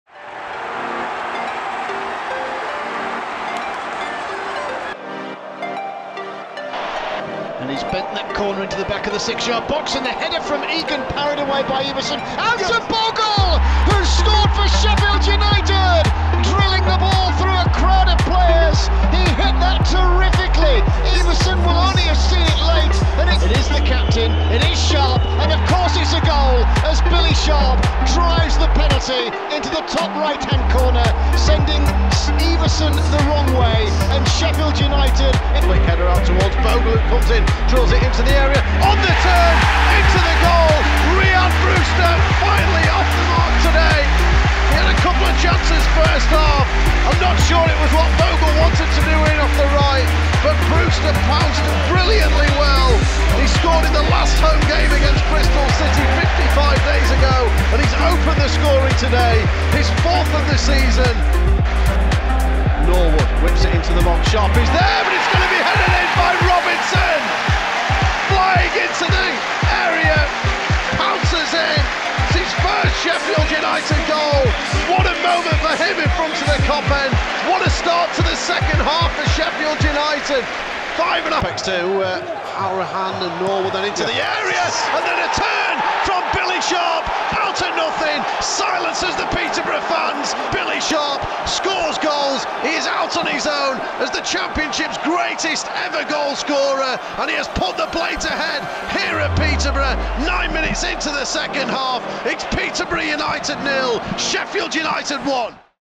Goal of the Month | Sheffield United January 2022 | Sharp, Brewster, Bogle, Robinson Goals
Voting is now open for Sheffield United's January 2022 'Goal of the Month'. Leave your vote in the comments section.

A) Jayden Bogle vs Preston
B) Billy Sharp vs Preston
C) Rhian Brewster vs Luton
D) Jack Robinson vs Luton
E) Billy Sharp vs Peterborough